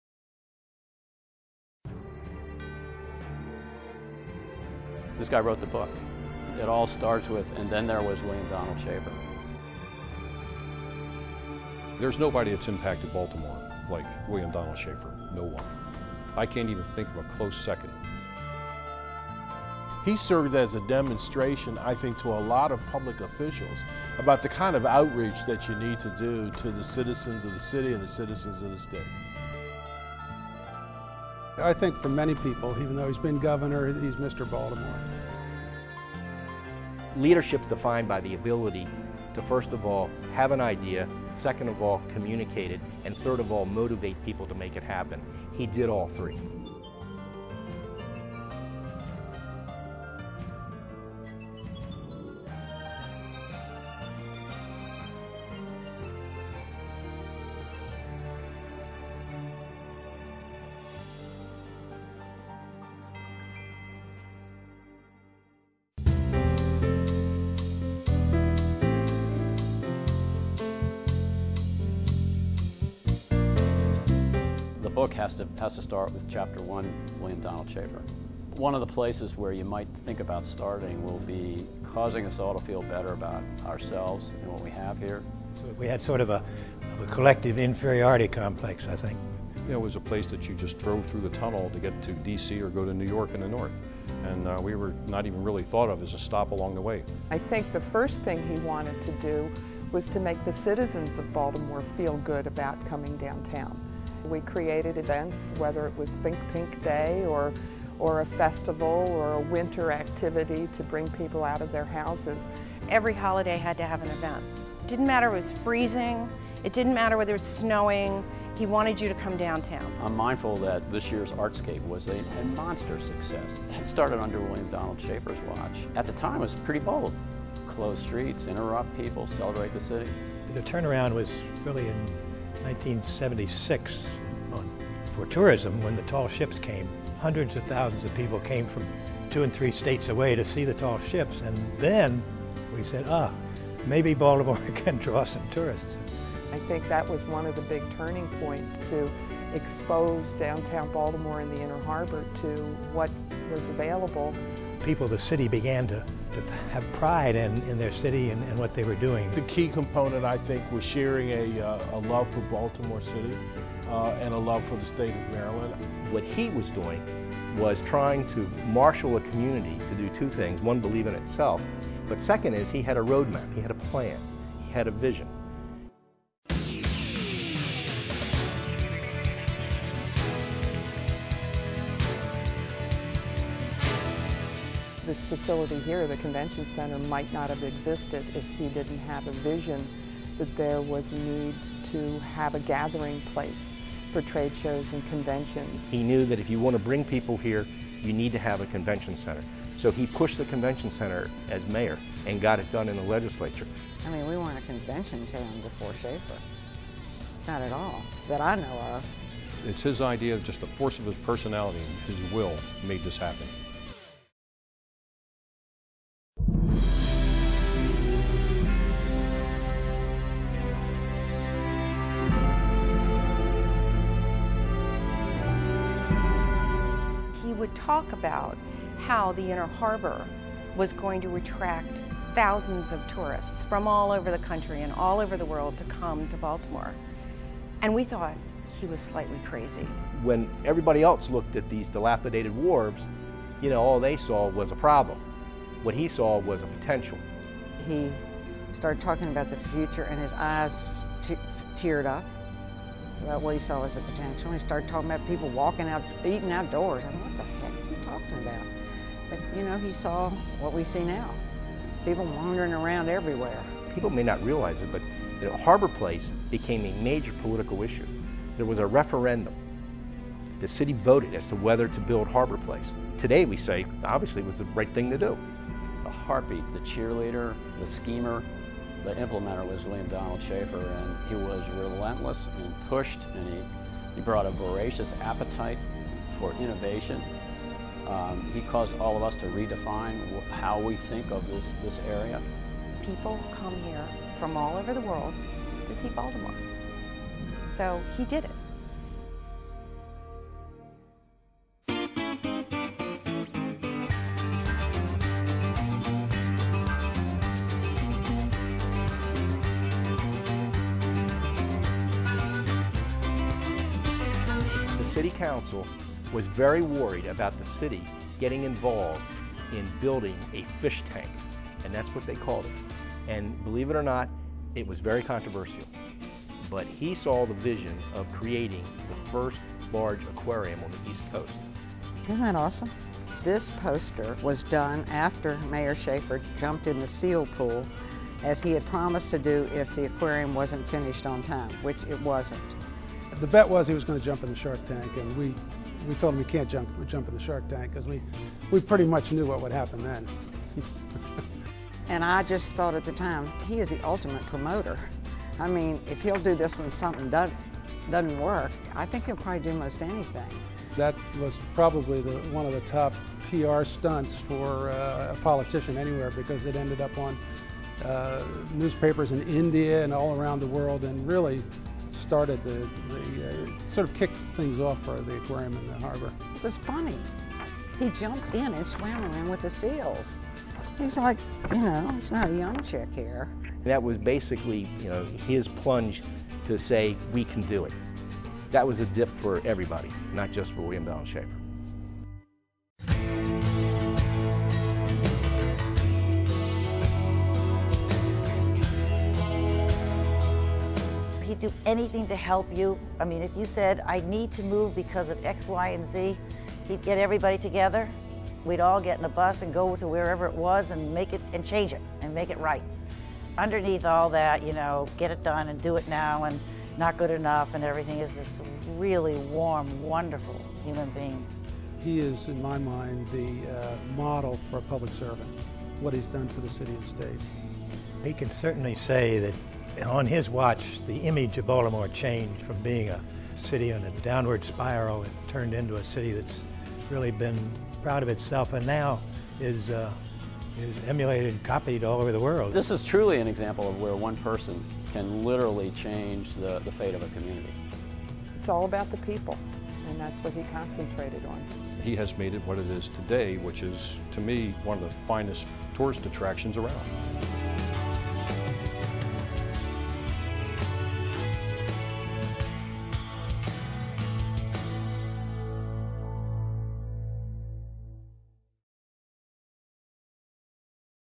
Honoring William Donald Schaefer
A tribute to former Governor and Comptroller of Maryland and Mayor of Baltimore in a look at his achievements as a Baltimore tourism visionary.